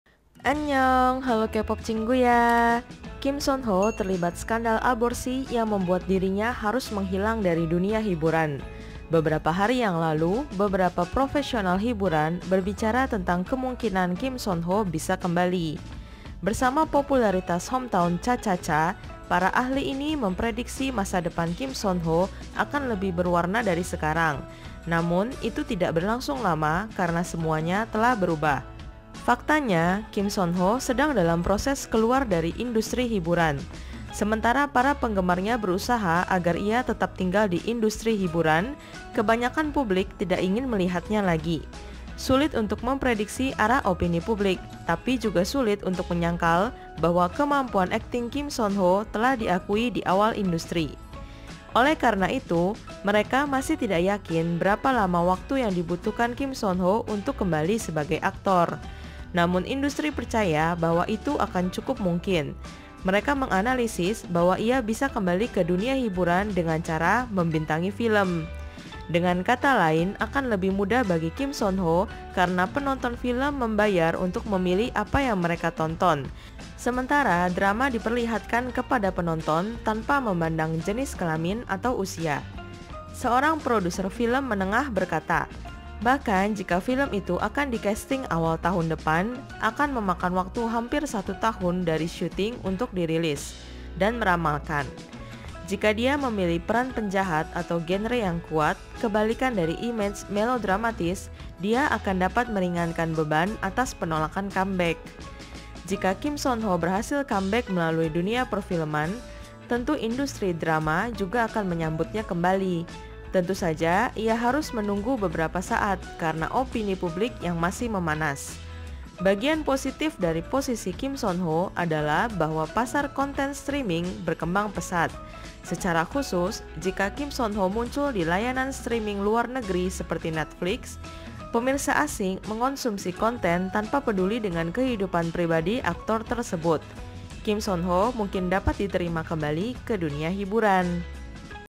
KIM SEON HO BISA BALIK KE DUNIA HIBURAN LEWAT CARA DARI PARA AHLI INI MESKI TERJERAT SKANDAL HEBOH!!
Kim Seon Ho terlibat skandal aborsi yang membuat dirinya harus menghilang dari dunia hiburan. Beberapa hari yang lalu, beberapa profesional hiburan berbicara tentang kemungkinan Kim Seon Ho bisa kembali.
Bersama popularitas "Hometown Cha-Cha-Cha", para ahli ini memprediksi masa depan Kim Seon Ho akan lebih berwarna dari sekarang. Namun, itu tidak berlangsung lama karena semuanya telah berubah.
Faktanya, Kim Seon Ho sedang dalam proses keluar dari industri hiburan. Sementara para penggemarnya berusaha agar ia tetap tinggal di industri hiburan, kebanyakan publik tidak ingin melihatnya lagi. Sulit untuk memprediksi arah opini publik, tapi juga sulit untuk menyangkal bahwa kemampuan akting Kim Seon Ho telah diakui di awal industri.
Oleh karena itu, mereka masih tidak yakin berapa lama waktu yang dibutuhkan Kim Seon Ho untuk kembali sebagai aktor. Namun industri percaya bahwa itu akan cukup mungkin. Mereka menganalisis bahwa ia bisa kembali ke dunia hiburan dengan cara membintangi film.